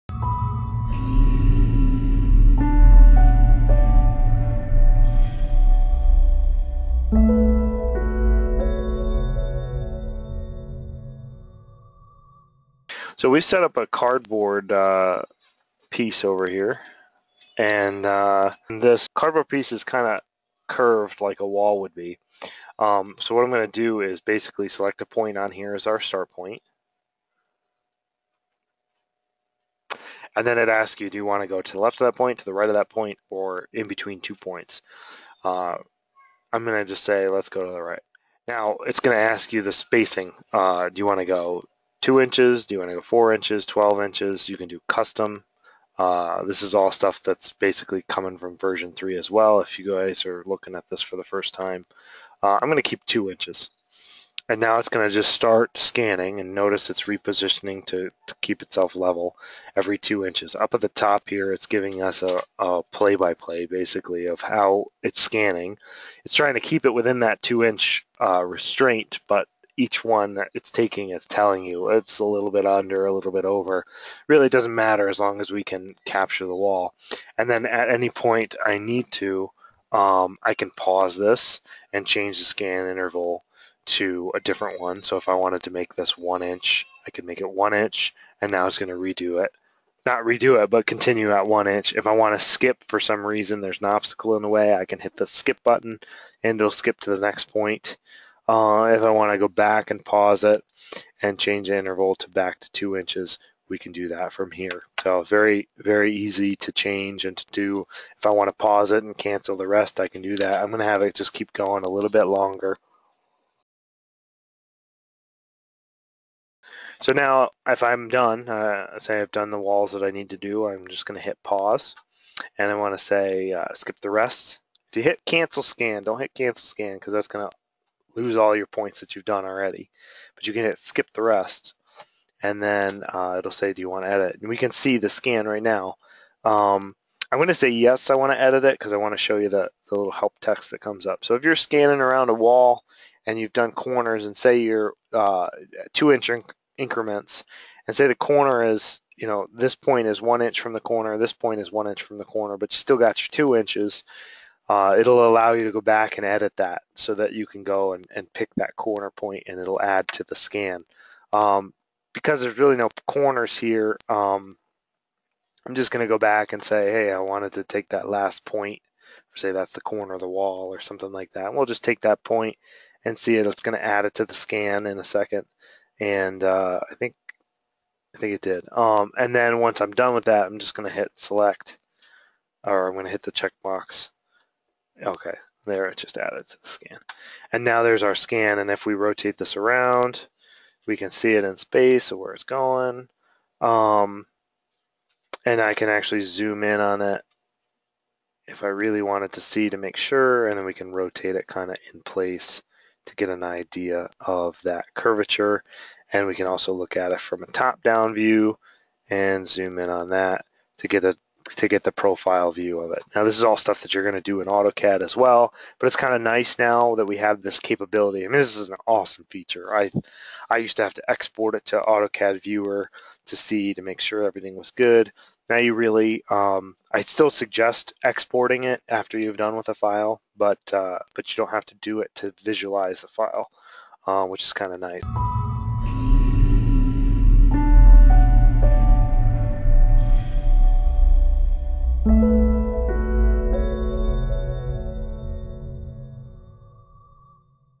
Using the Autoscan Feature on the Leica 3D Disto v4.0
The Leica 3D Disto's autoscan feature is handy to use especially when templating irregularly shaped objects or if you need a quick, accurate 3D scan of a wall. Take a look and give us a call with any questions: